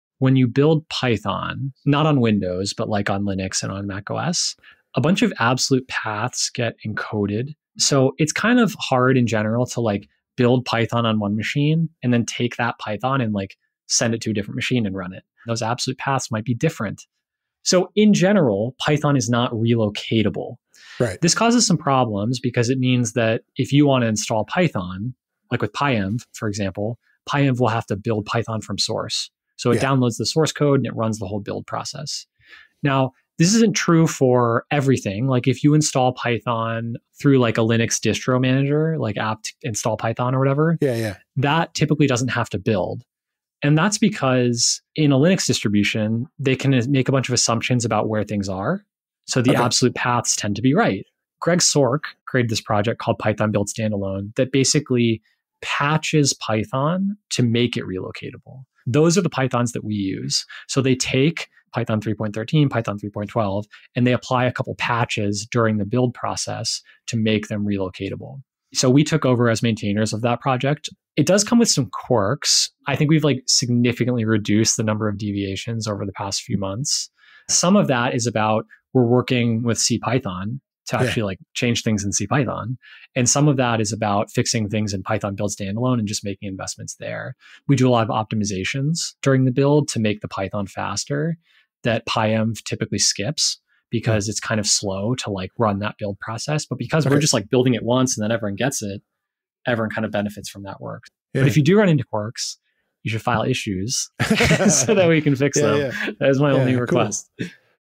Why Python Isn’t Relocatable -- and How to Fix It
From our podcast, episode 238 with Charlie Marsh (hosted by Chris Bailey).